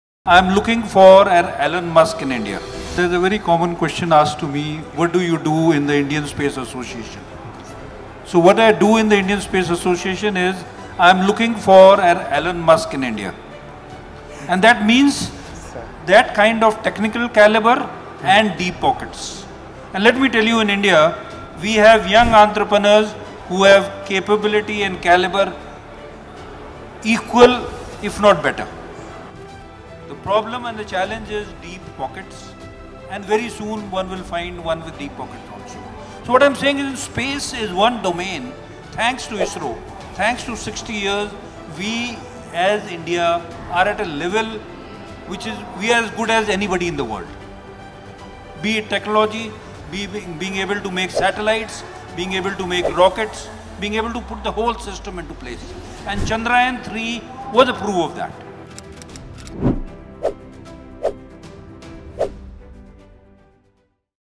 "I am looking for an Elon Musk in India" | Lt Gen AK Bhatt at Convergence India Expo 2025
Lt Gen Anil Kumar Bhatt (retd) , DG, ISpA- Indian Space Association, made this powerful remark during a session, spotlighting India’s potential, caliber, and the missing link that could spark our space-tech revolution.

Watch the full session now on our YouTube channel and join the conversation shaping the future of India’s space journey.